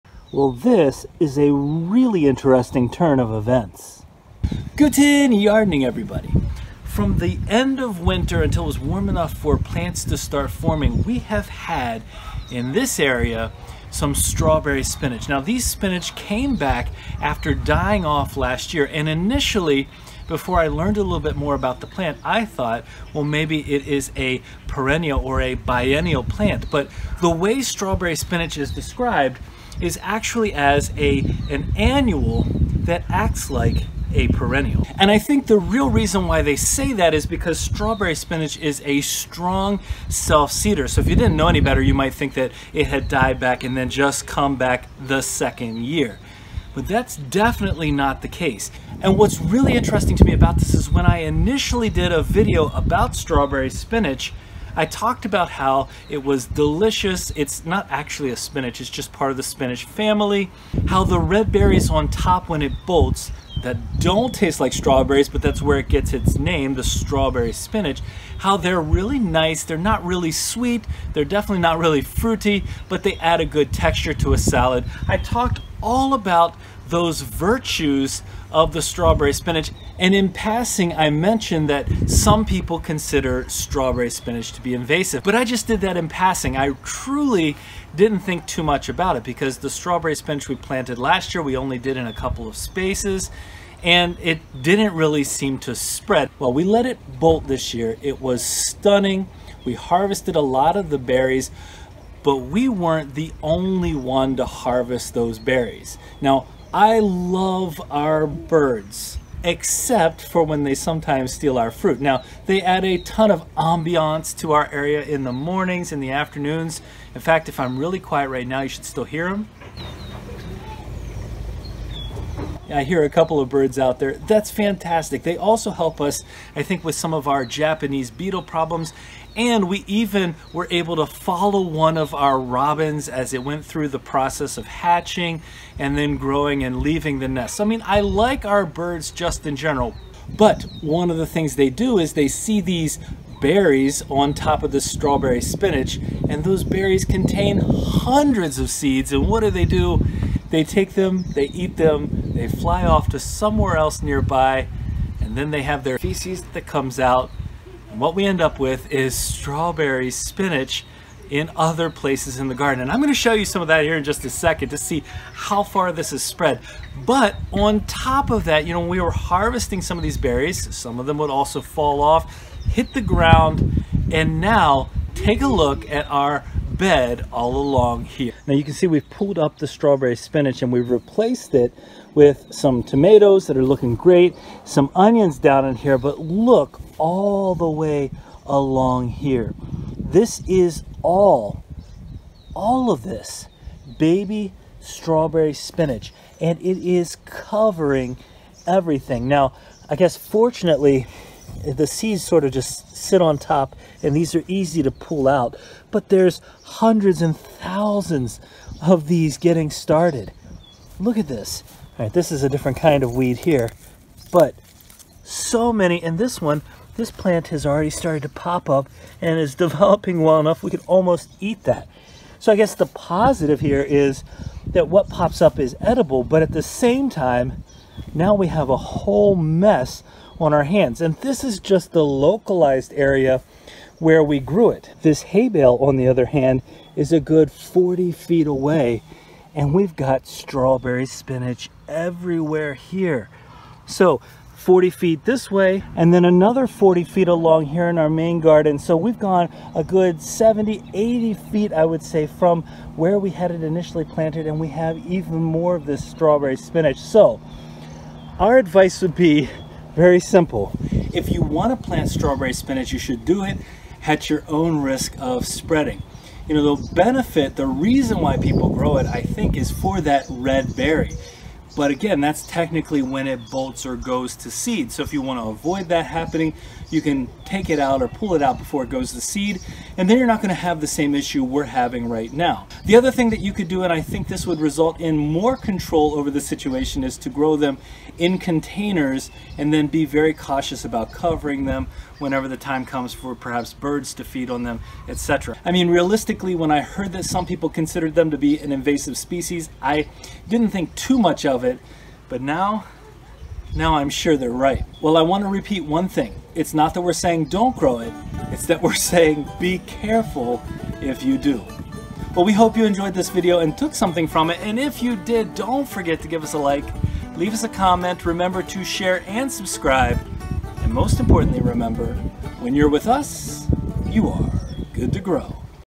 Growing Strawberry Spinach | Gardening 101 | Guten Yardening
Beautiful! Delicious! Invasive? | Growing Strawberry Spinach | Gardening 101 | Guten Yardening

We absolutely love the look of our strawberry spinach plants.  We first planted them last year, and this year, we had an abundant supply of the beautiful strawberry spinach berries.

This year, the strawberry spinach plants appeared right after the winter finished in about 5 or 6 places where we had planted them last year.  Since this was our first time growing them, we had a lot to learn about what this meant.

In this video, we talk about what we learned from growing the strawberry spinach this year, and we provide a warning about growing this variety of vegetable.  While it is delicious, and the strawberry spinach berries are delicious on salad, we want to make sure you know what you are potentially getting into if you decide to grow this vegetable.

If you like my hat, you can get it

Join our public Facebook group: Search Guten Yardening